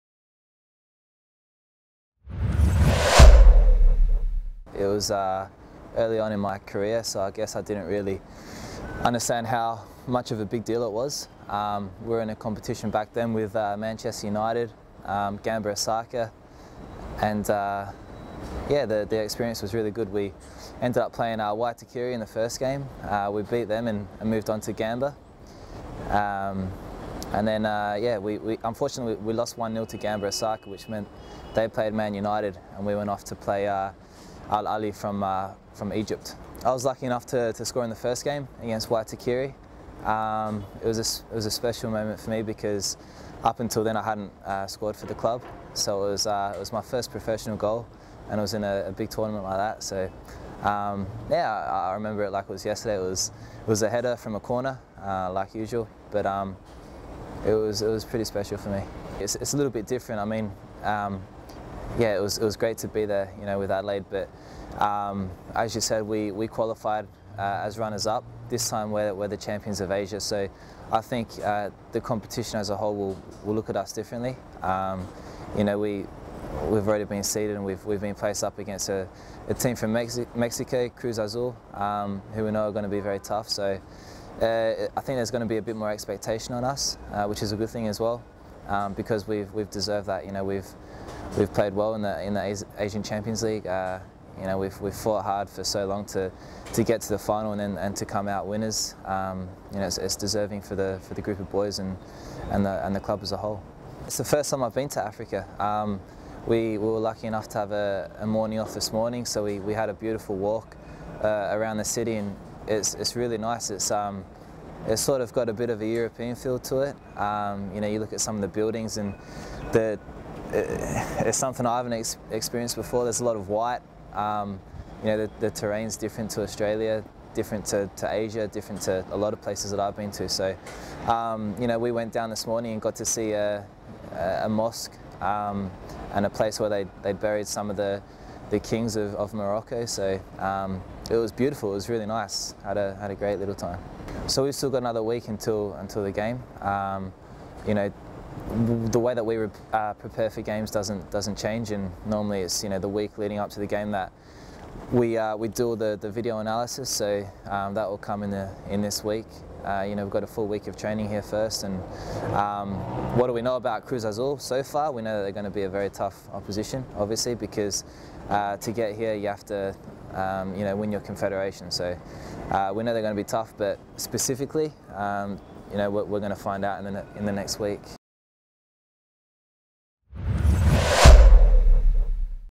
Club World Cup | Mullen on his Club World Cup history
Daniel Mullen chats to WSW TV about his previous experience in the FIFA Club World Cup